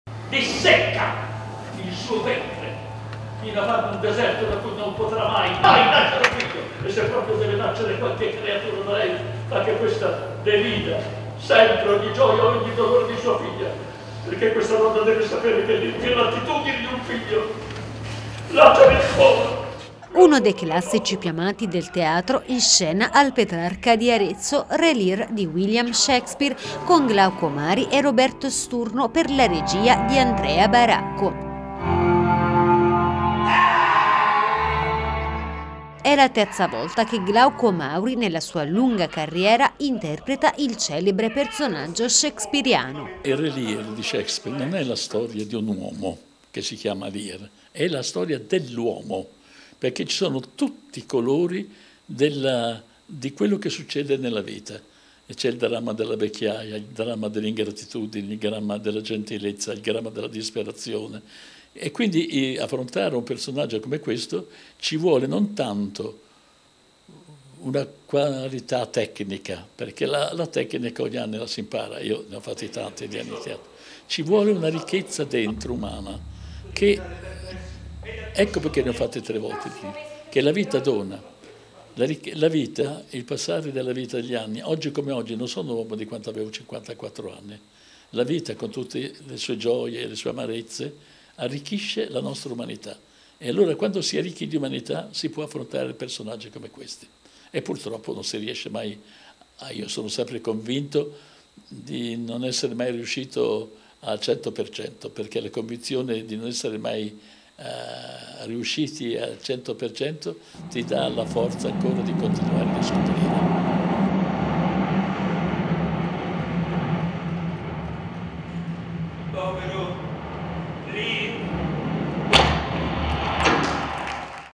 Al Teatro Petrarca "Re Lear" di William Shakespeare, con Glauco Mauri e Roberto Sturno
Uno dei classici più amati del teatro in scena al Teatro Petrarca: "Re Lear", di William Shakespeare, con Glauco Mauri e Roberto Sturno per la regia di Andrea Baracco. È la terza volta, nella sua lunga carriera, che Mauri interpreta il celebre personaggio shakespeariano.